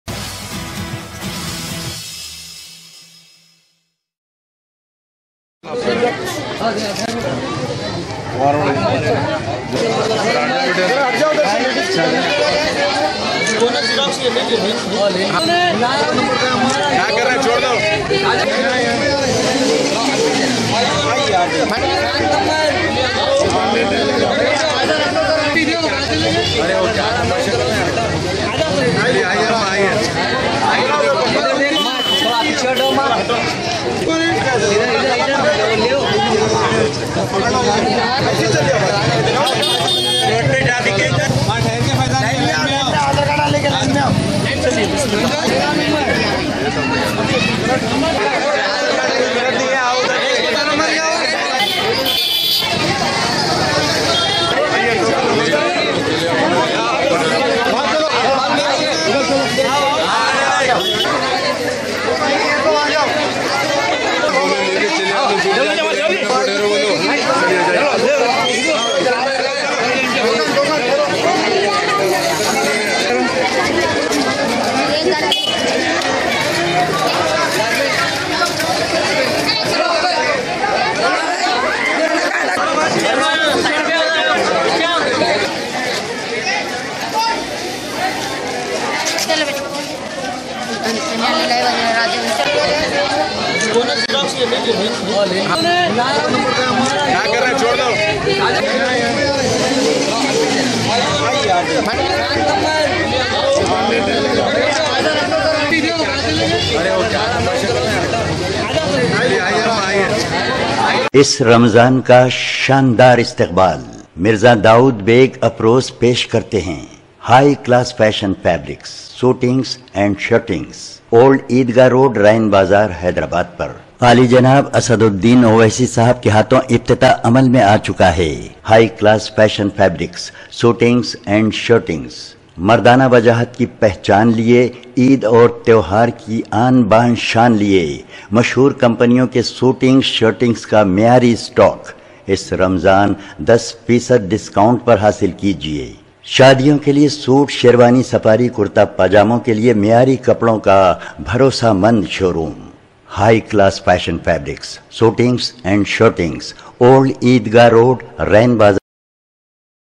MIM Corporator Md Mustafa Ali Muzafar Distributed Clothes
MIM Corporator From ShaliBanda Mohammed  Mustafa Ali Muzafar Distributed Clothes among   Poor in Shakar Gunj Kumar wadi Hyderabad on 19th June